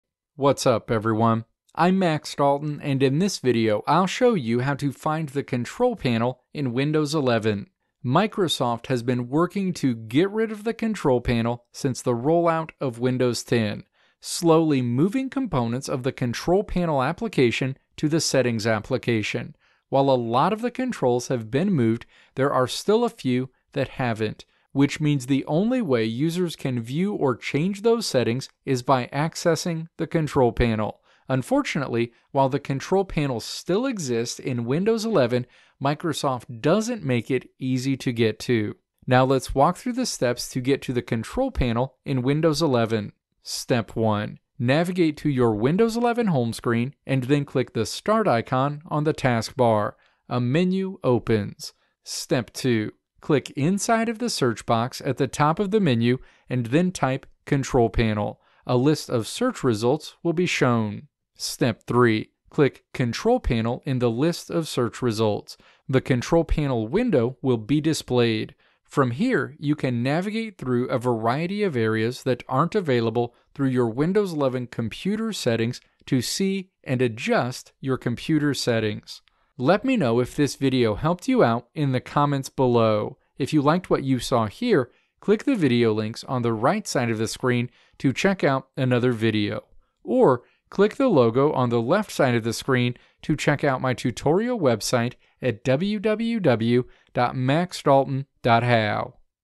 How to Find Control Panel in Windows 11
In this video I'll show you how to find the Control Panel in Windows 11.

Timestamps:
Introduction: 0:00
Steps to Find Control Panel in Windows 11: 0:38
Conclusion: 1:19

Video Transcript:
Microsoft has been working to get rid of the Control Panel since the rollout of Windows 10, slowing moving components of the Control Panel application to the Settings application. While a lot of the controls have been moved, there are still a few that haven't, which means the only way users can view or change those settings is by accessing the Control Panel. Unfortunately, while the Control Panel still exists in Windows 11, Microsoft doesn't make it easy to get to.

Now let's walk through the steps to get to the Control Panel in Windows 11.

Step 1. Navigate to your Windows 11 home screen, and then click the "Start" icon on the taskbar. A menu opens.

Step 2. Click inside of the search box at the top of the menu, and then type "Control Panel." A list of search results will be shown.

Step 3. Click "Control Panel" in the list of search results. The Control Panel window will be displayed. From here you can navigate through a variety of areas that aren't available through your Windows 11 computer's settings to see and adjust your computer's settings.